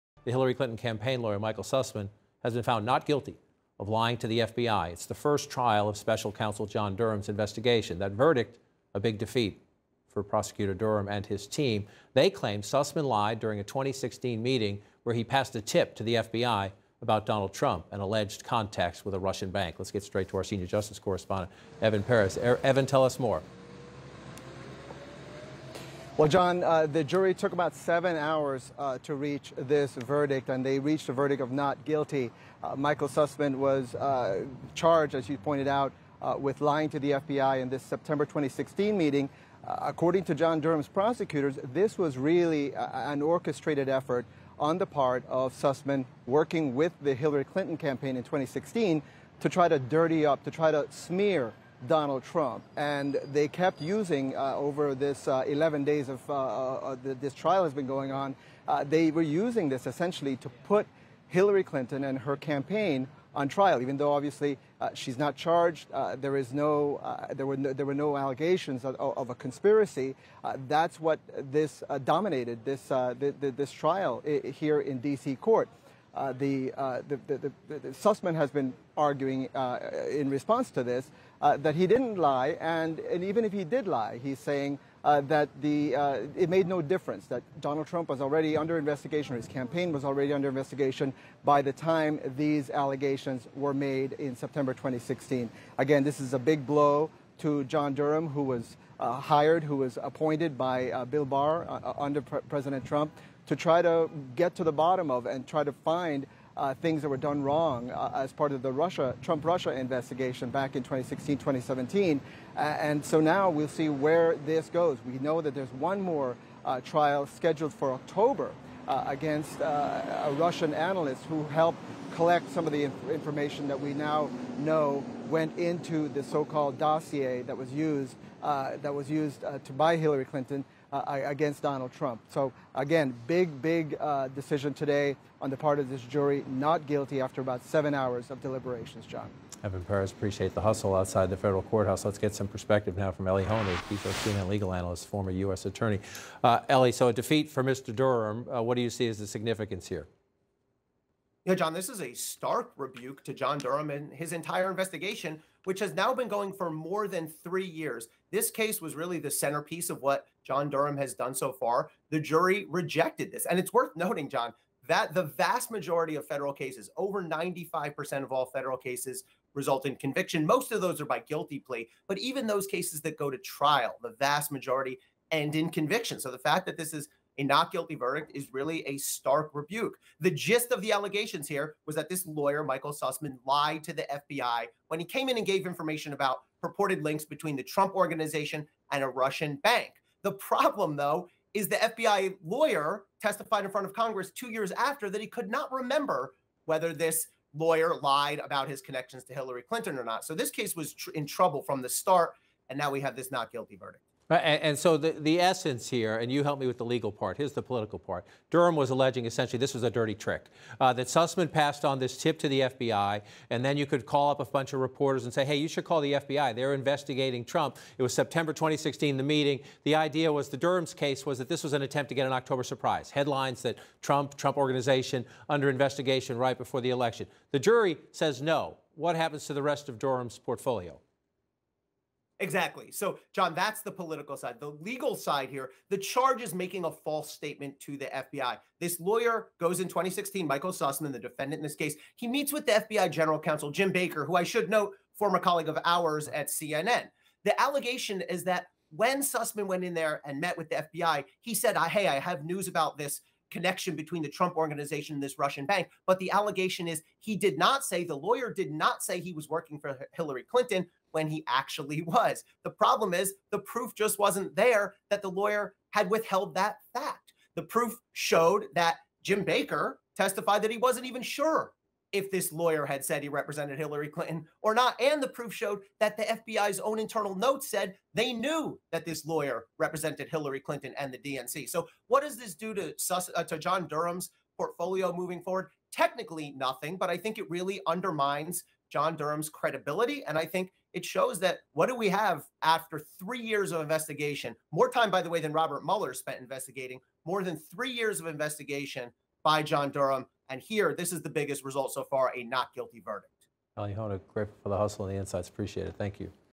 ‘Stark rebuke’: Clinton campaign lawyer acquitted of lying to FBI
Hillary Clinton campaign lawyer Michael Sussmann was acquitted of lying to the FBI, in the first trial of special counsel John Durham's investigation. The verdict is a major defeat for Durham and his Justice Department prosecutors, who have spent three years looking for wrongdoing in the Trump-Russia probe.